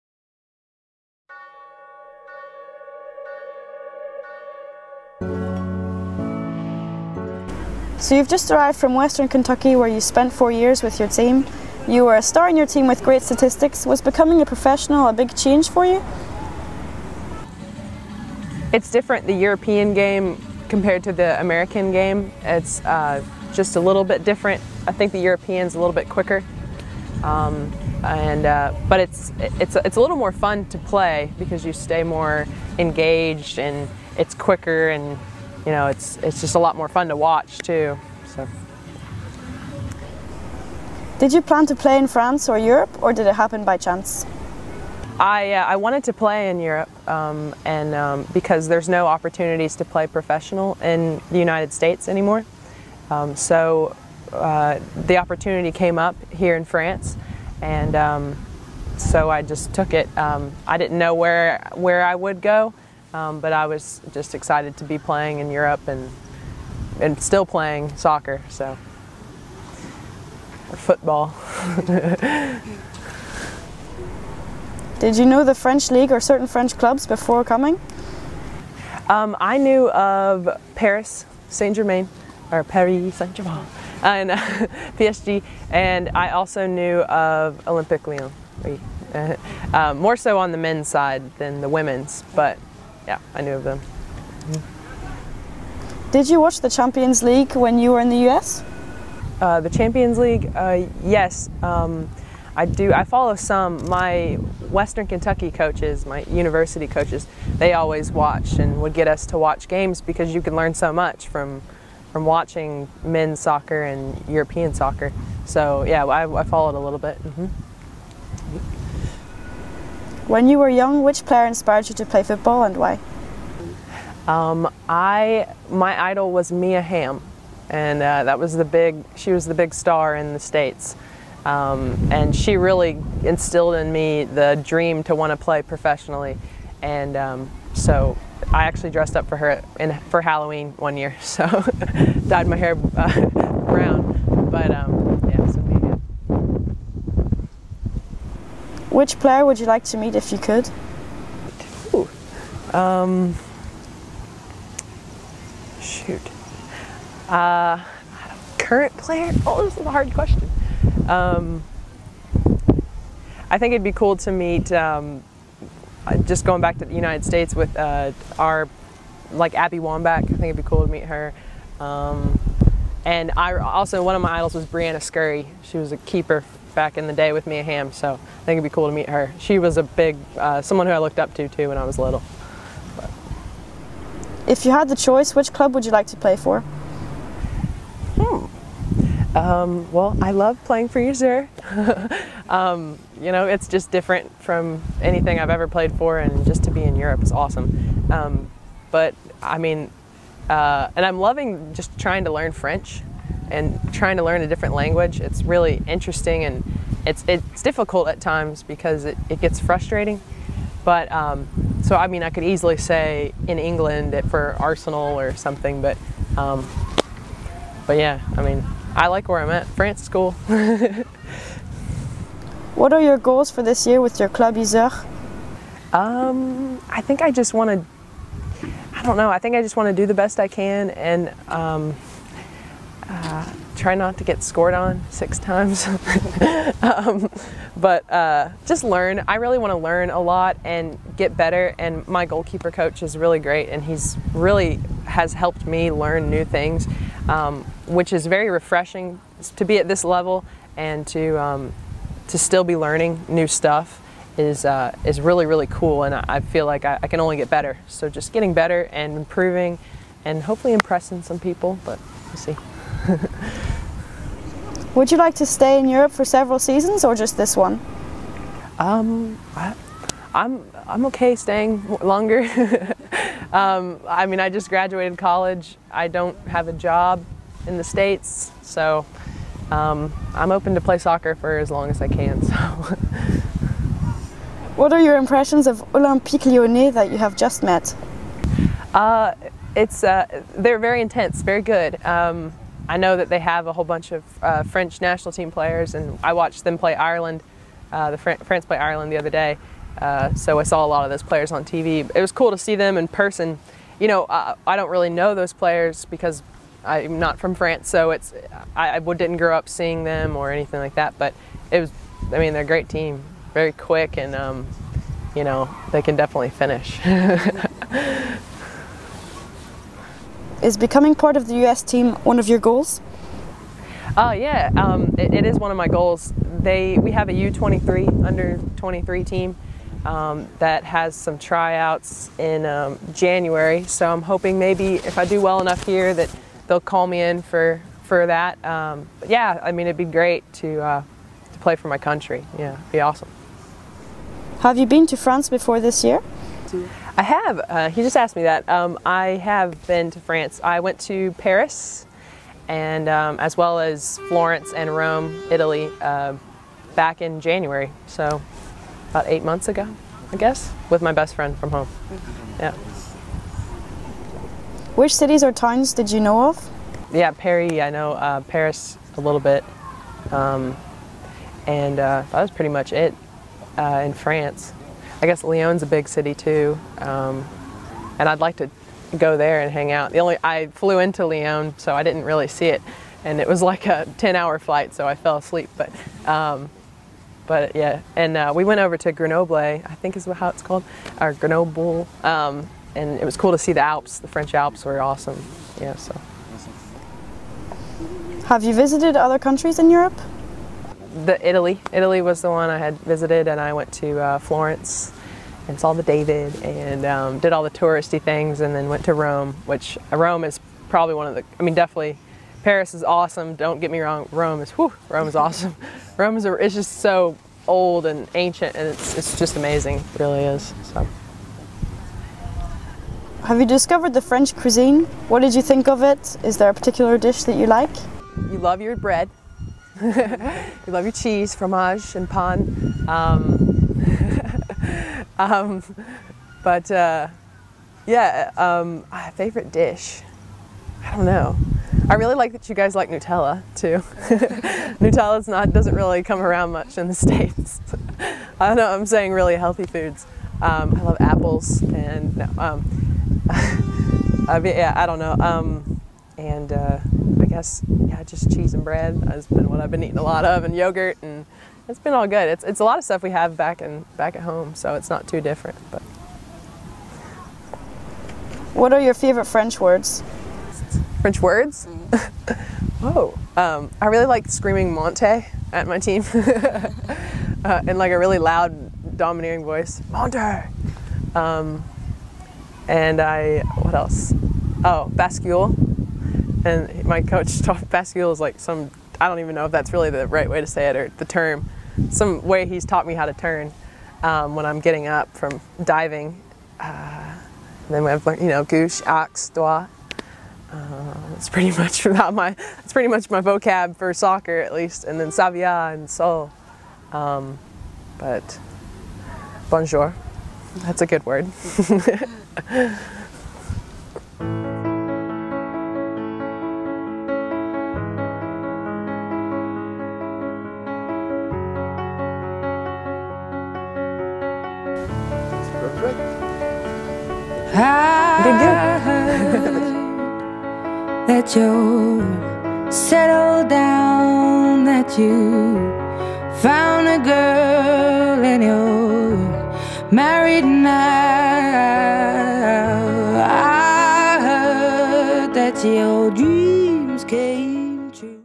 Interview Elisabeth "Libby" Stout Goalkeeper Yzeure le 23/09/2012
Interview de Elisabeth Stout goalkeeper de Yzeure par Romain Biard le 23/09/2012